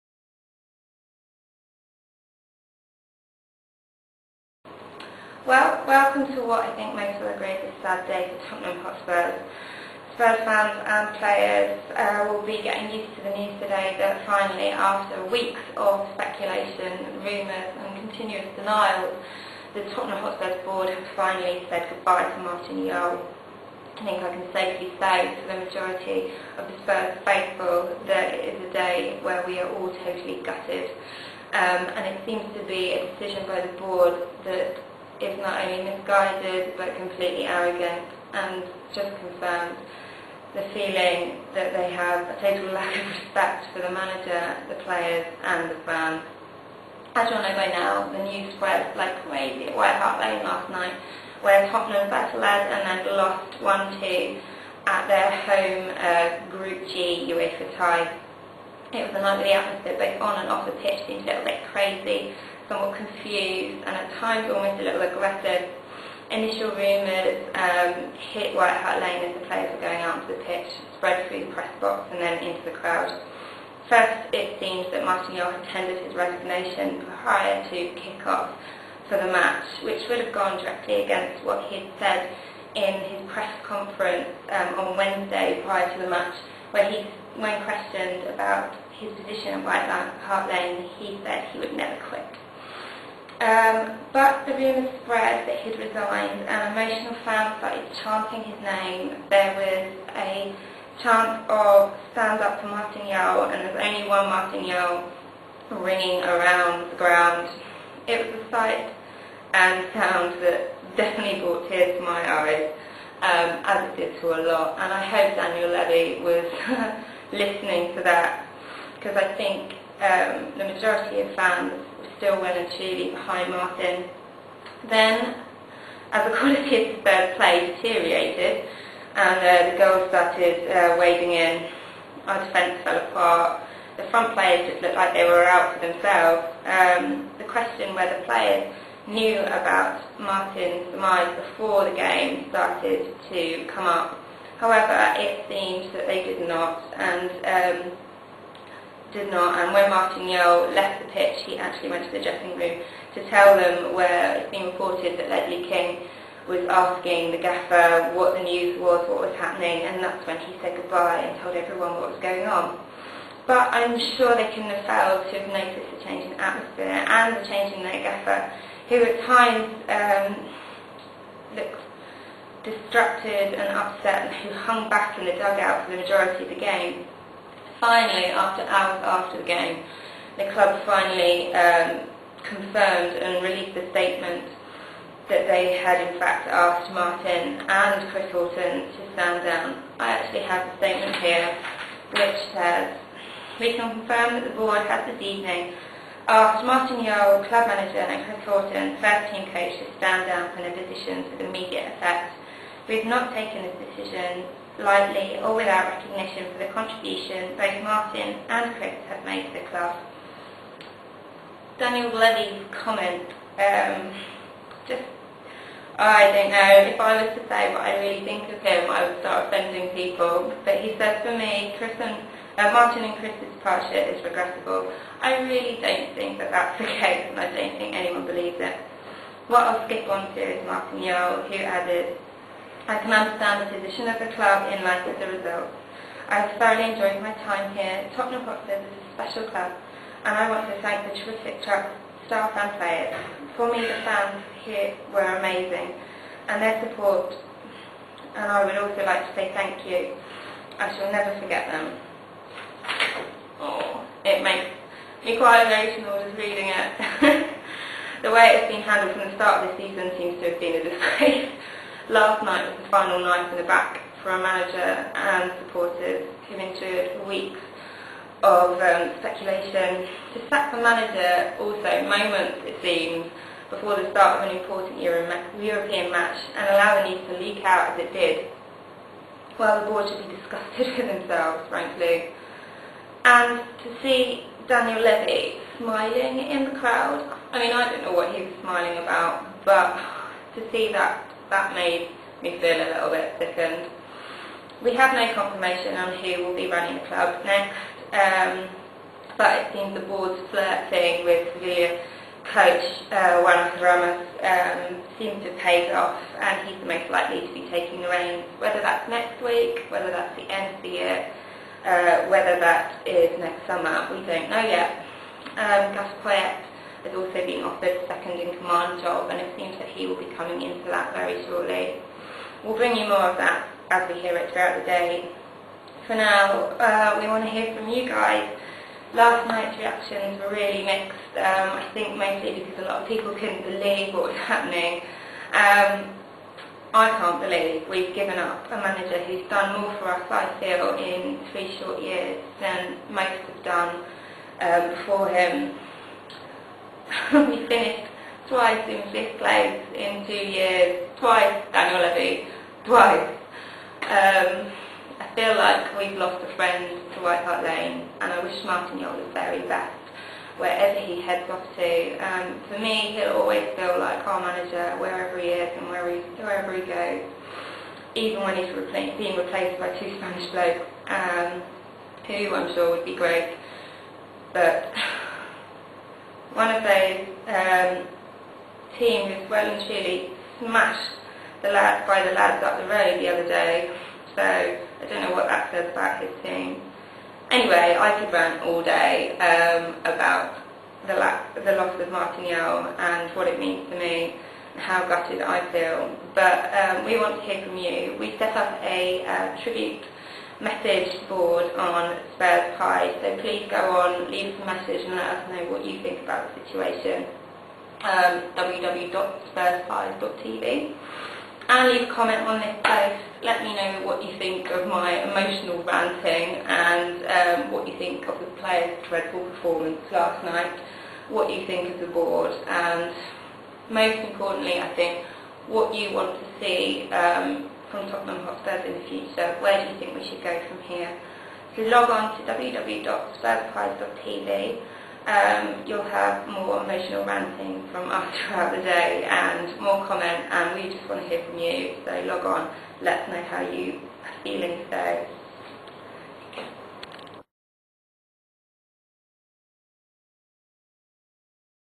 Zoe is The Hot Spur - Goodbye Martin Jol
Martin Jol has left the Lane... Tottenham Hotspur say goodbye to Martin Jol, who leaves along with Chris Hughton. In my eyes he'll always be a legend of the Lane. Here are my thoughts on last night's news and events - Brought to you - the best blog on the web for news and views about Tottenham Hotspur.